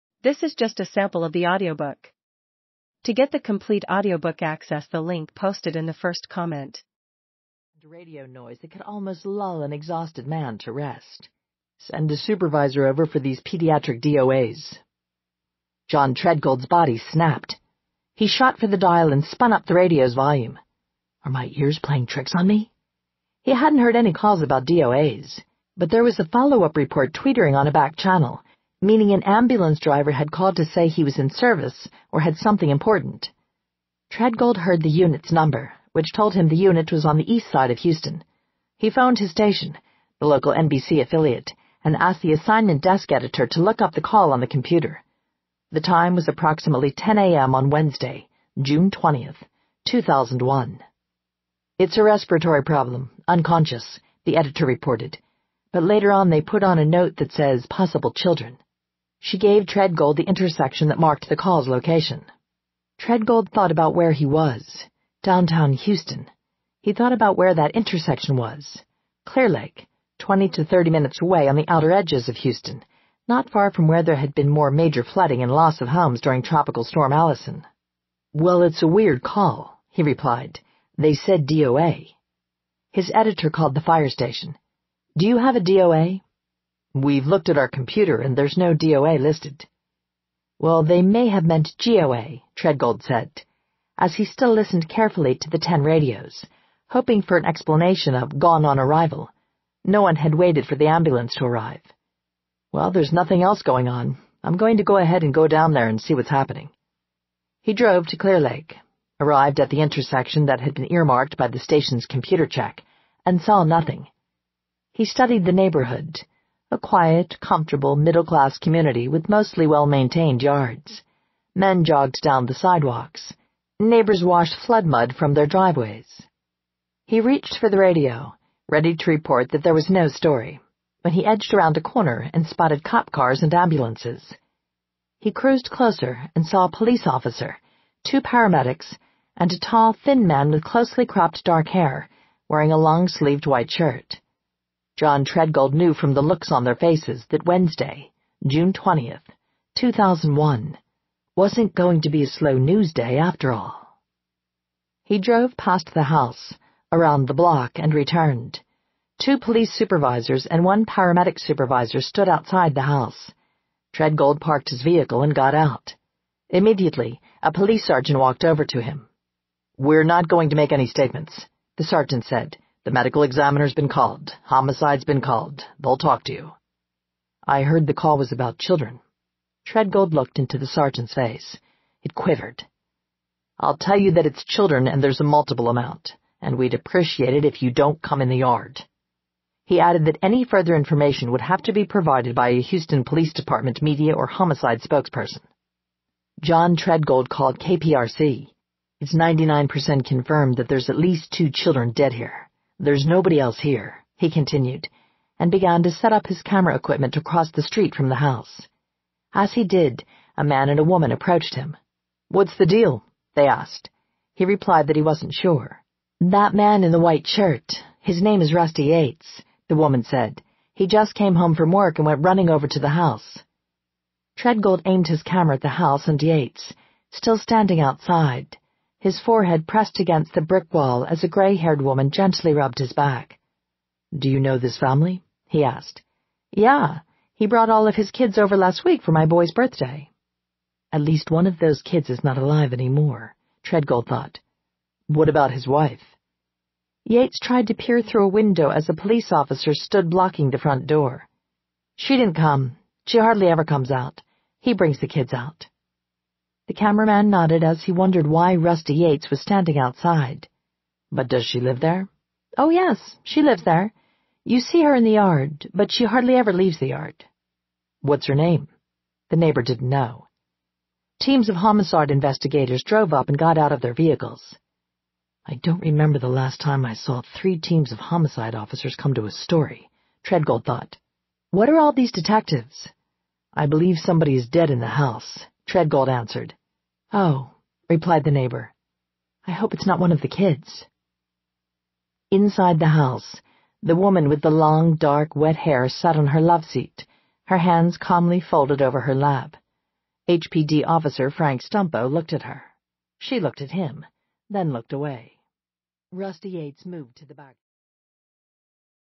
Breaking Point | Audiobook Sample
Breaking Point

While her husband Rusty, a NASA engineer, was at work, Andrea Yates filled the family bathtub with water and systematically drowned their children, ages six months to seven years. As her eldest child lay lifeless in the bathtub and the bodies of her four youngest rested in her bed, Andrea, a devoted Christian wife and former nurse, called the police to confess her sin. The investigations by the prosecutors, by the defense, and by the press delved deeper into Andrea's mind and history, revealing a disturbing web of suicidal tendencies, depression, and psychoses. While her husband struggled between his overwhelming grief and his loyalty to his incarcerated wife, an outraged nation asked one question after another: How could anyone do this? What would drive a mother to kill her children?